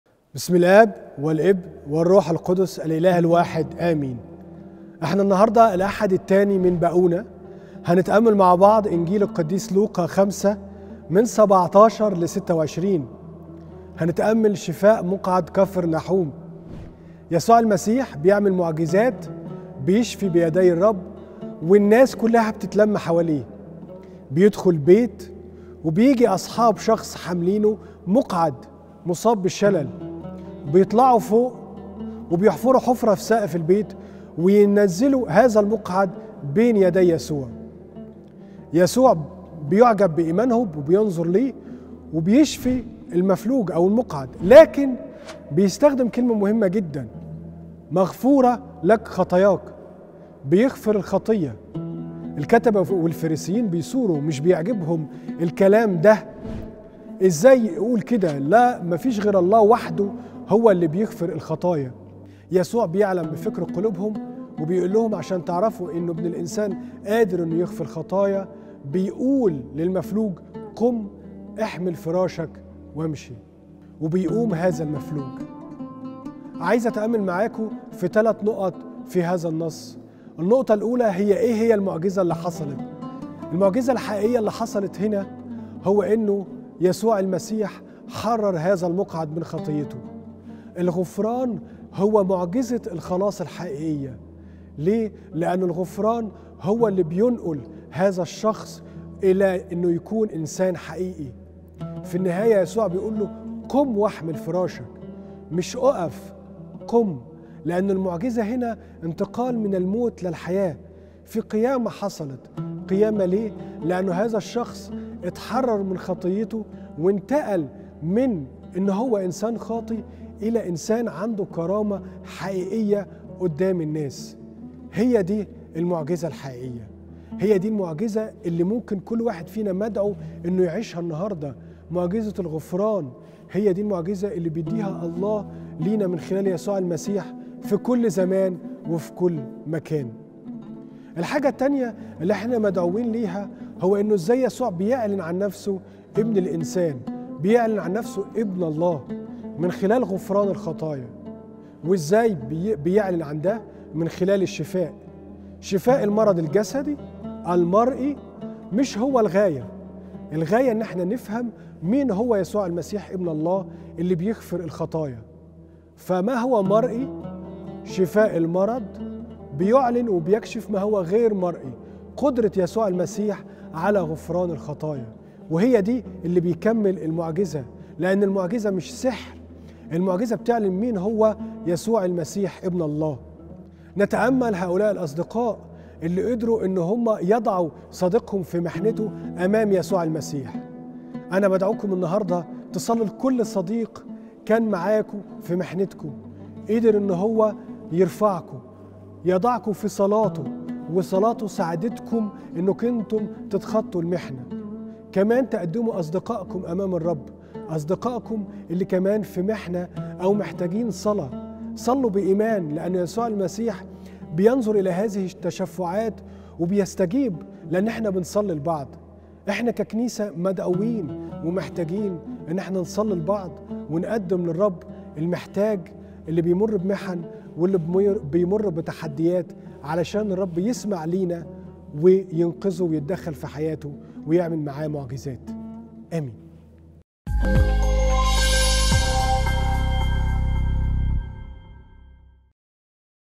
مش هطول عليك | الصديق
الأب يوسف عبد النور اليسوعي ـ شفاء مقعد كفر ناحوم

الاحد الثاني  من بؤونة لو5: 17-٢٦

برنامج مش هطول عليك كل أسبوع عظة قراءة الإنجيل المقدس في أقل من ٥ دقايق من طقس مختلف و كاهن مختلف